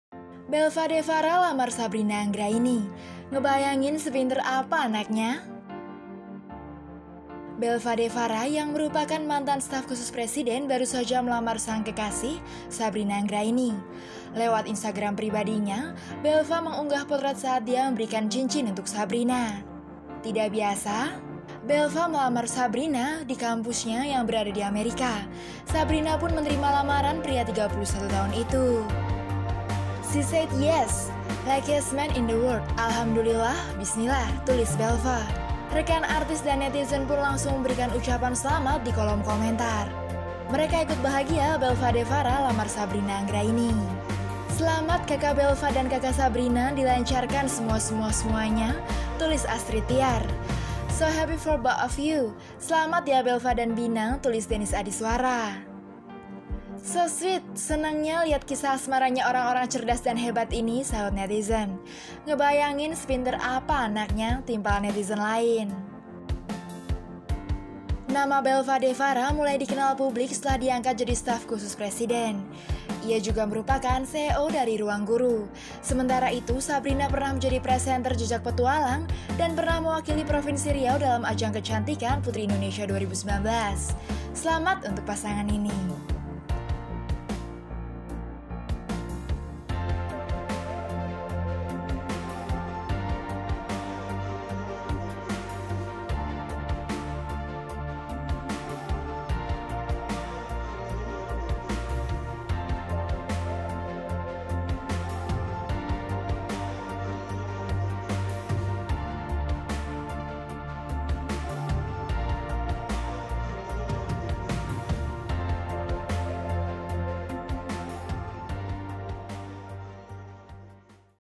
Belva Devara Lamar Sabrina Anggraini: Ngebayangin Sepinter Apa Anaknya!
Belva Devara yang merupakan mantan staf khusus Presiden baru saja melamar sang kekasih, Sabrina Anggraini. Lewat Instagram pribadinya, Belva mengunggah potret saat dia memberikan cincin untuk Sabrina.

Tak biasa, Belva melamar Sabrina di depan kampus Massachusetts Institute of Technology yang berada di Amerika. Sabrina pun menerima lamaran pria 31 tahun itu.

Pengisi Suara / Video Editor: Anistya / Ariskha Ridhal Ikhrom